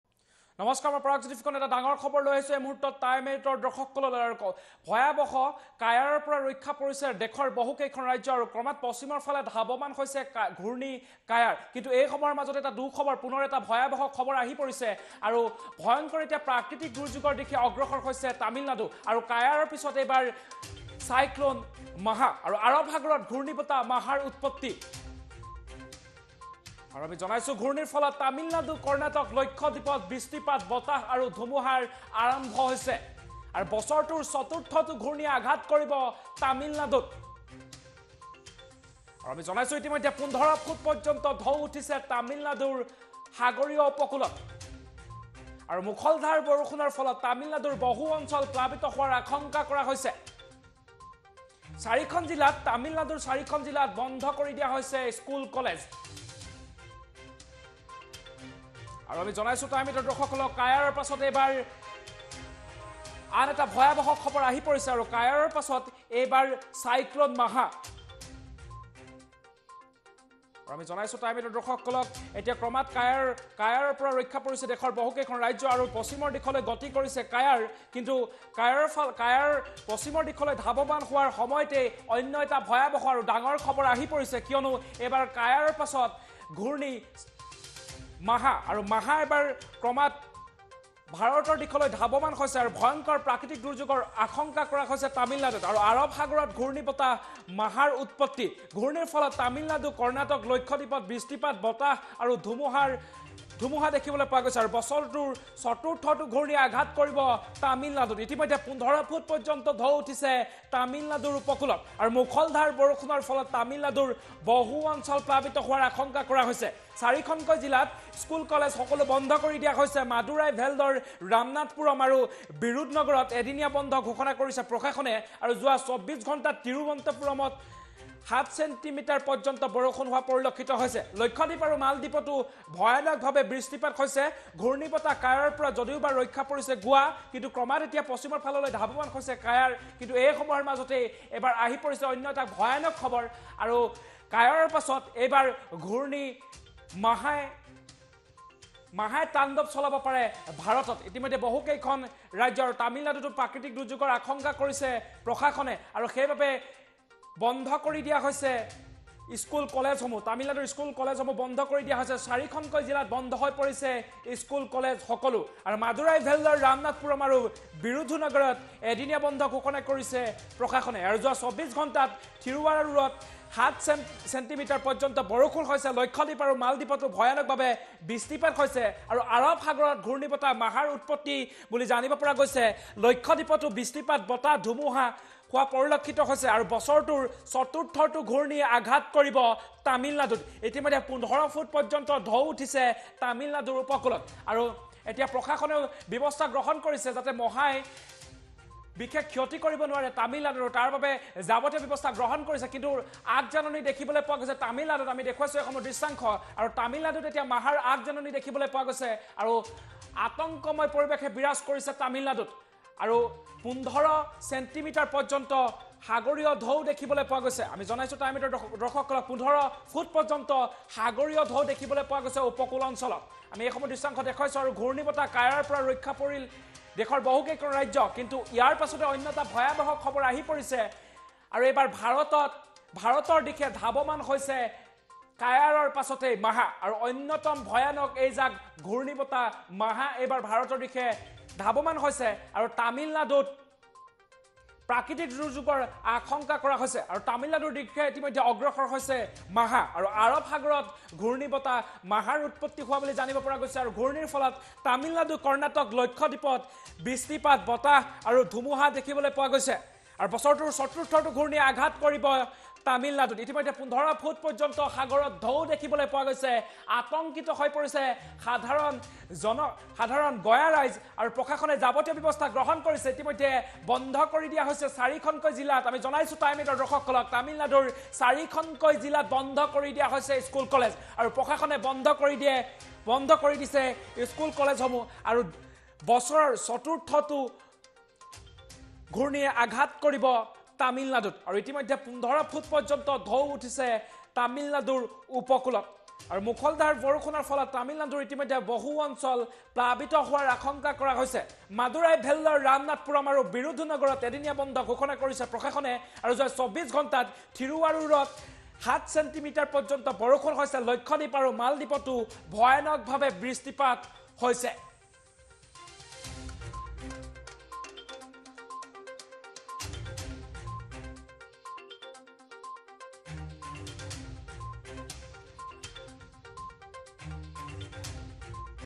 NEWS - ধুমুহাই কঁপাব দেশ, তছনছ কৰিব পাৰে কেইবাখনো চহৰ, আহি আছে ঘূৰ্ণী বতাহ মাহা
ভয়ংকৰ ধুমুহাই কঁপাব দেশ, তছনছ কৰিব পাৰে কেইবাখনো চহৰ, আহি আছে ঘূৰ্ণী বতাহ মাহা